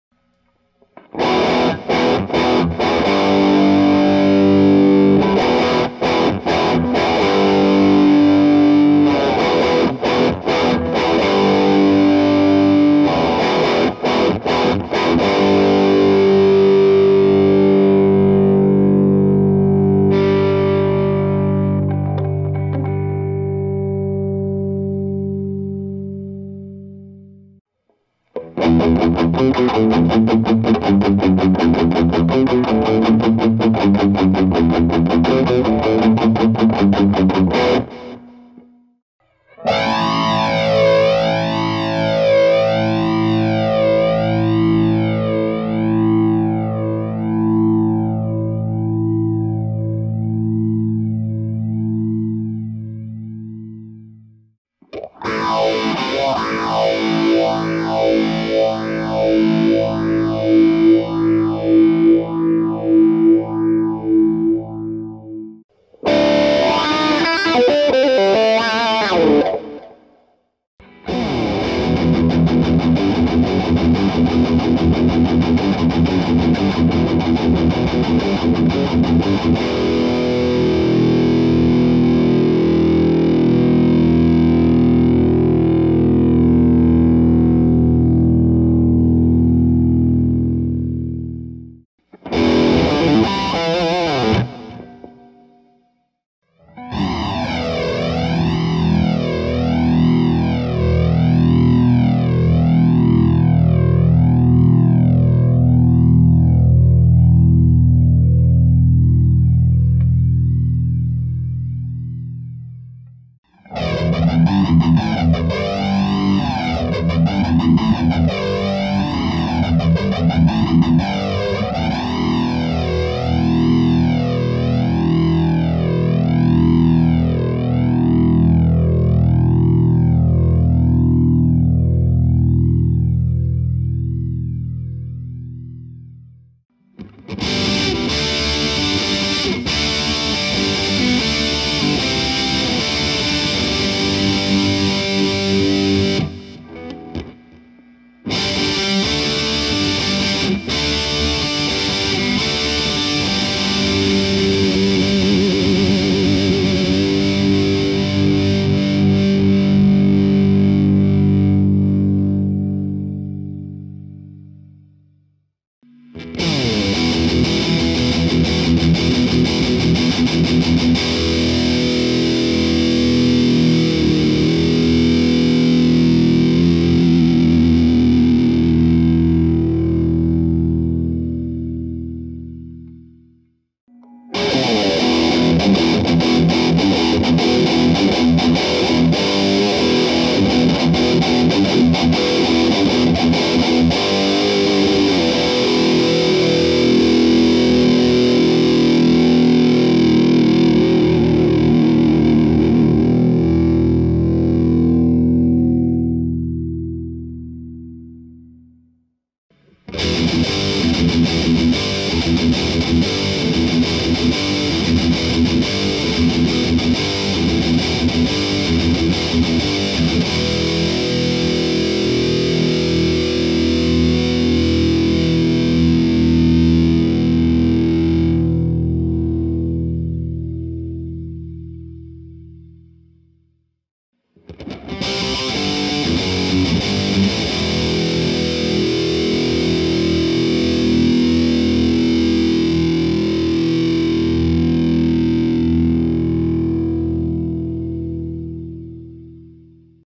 Royalty Free Guitar Sounds Vol. 3
Guitar riffs, licks, intros, and noises for your film or short film.